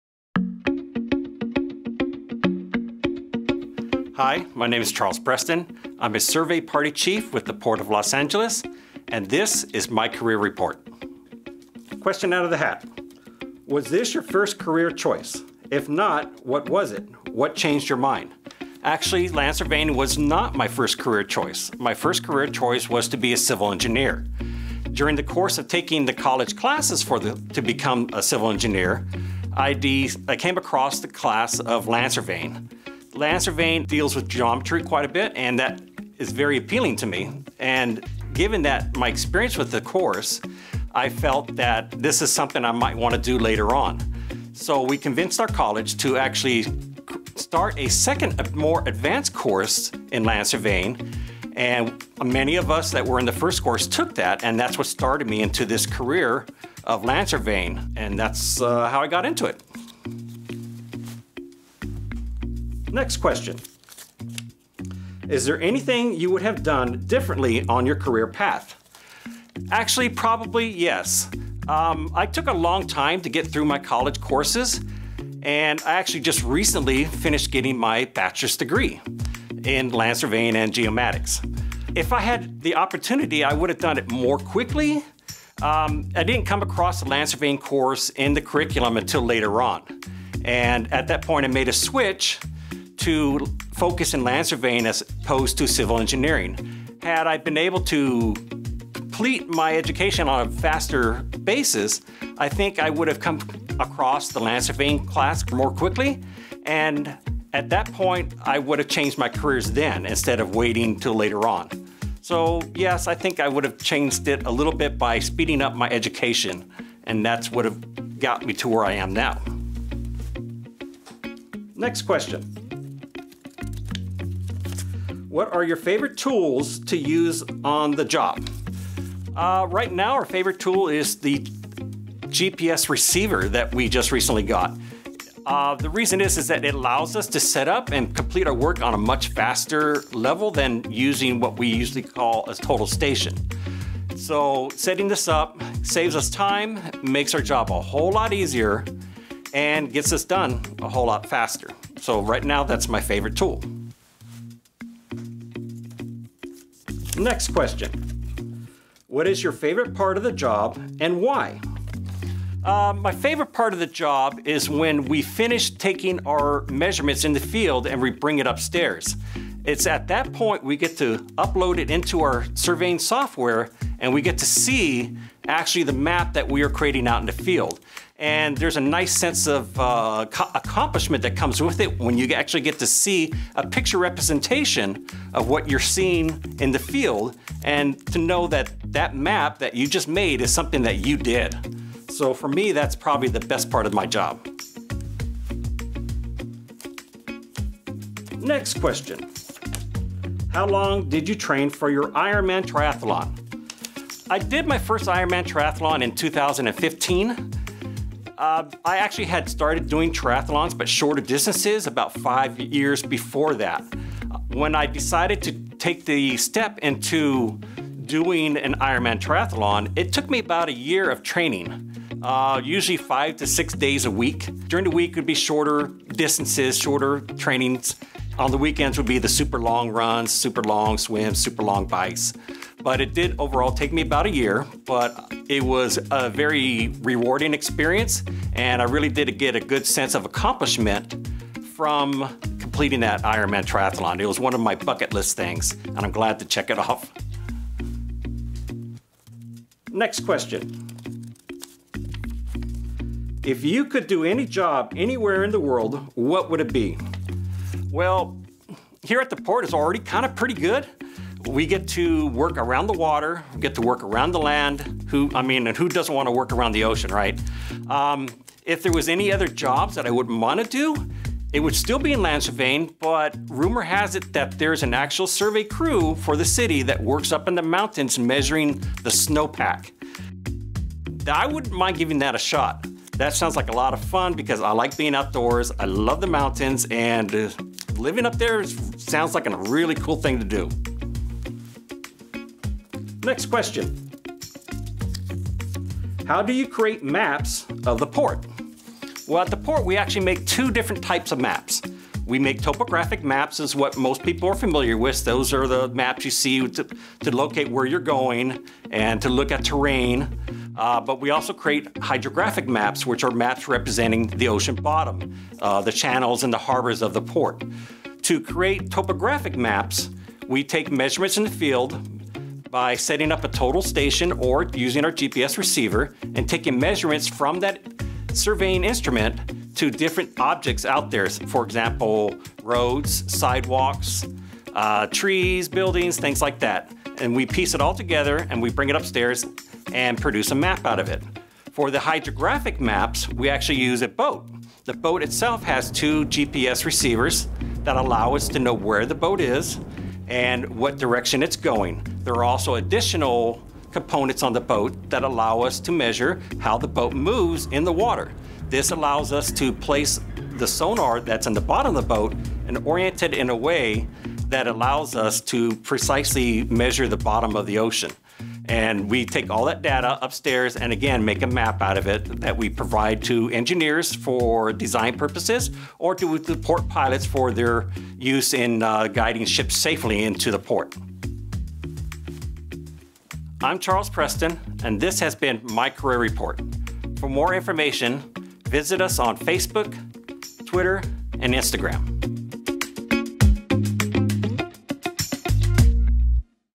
Port of Los Angeles: Career RePORT - Party Survey Edition
Career RePORT encourages all students and educators to submit questions you want to ask a Port of Los Angeles employee: what kind of job they do at the Port, how they entered their career field, who their favorite superhero is, or what they made for dinner last night.

This episode feature Charles Preston, Survey Party Chief. In his role, Charles:

- Creates maps of specific areas around the Port for engineering projects;
- Provides construction survey services, which includes staking water lines, storm drains, and curbs;
- Updates the Port's control point network using a combination of static GPS, RTK (Real Time Kinematic), and total station measurements.

Three fun facts about Charles:

- He completed his first Ironman triathlon, consisting of a 2.4 mile swim, 112 mile bike, and a 26.2 mile run, at age 45 and will hopefully do another at the end of 2021.
- His first career was actually testing and designing windows, patio doors, and curtain walls on buildings.
- He was a lifeguard for the City of Los Angeles after high school before quitting and then getting rehired as a field engineering aide seven years later.
The Port of Los Angeles is America’s Port®—the nation’s #1 container port and the global model for sustainability, security, and social responsibility.